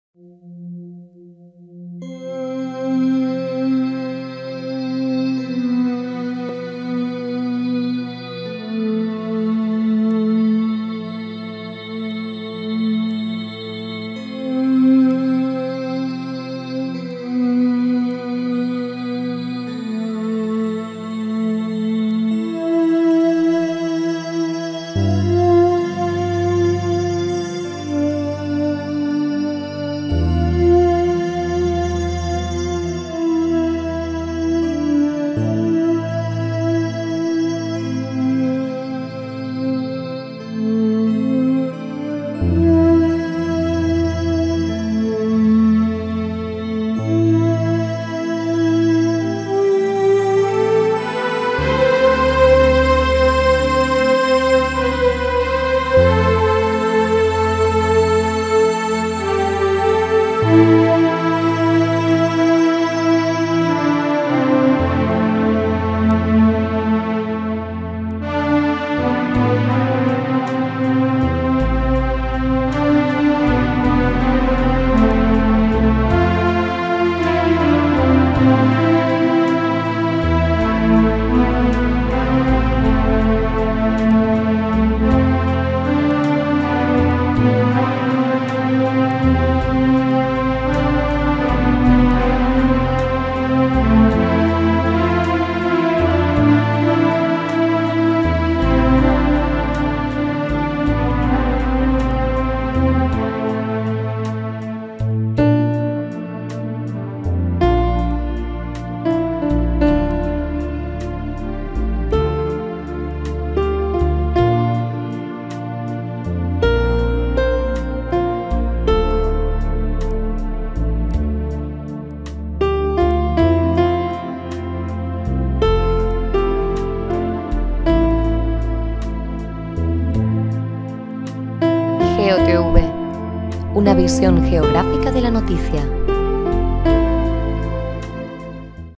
Planet X / Nibiru, Effects on Earth [IGEO TV]
The possible detection of a brown dwarf  ( called "Planet X or Nibiru ") by the space probes Pioneer 11 and Voyager 2 in 1992, leads us to the explanation of the consequences that its hypothetical step between the sun and Earth could have. Starting with a reversal of the poles, the movement of tectonic plates, large waves, and even strong air currents caused by the reversal of the poles.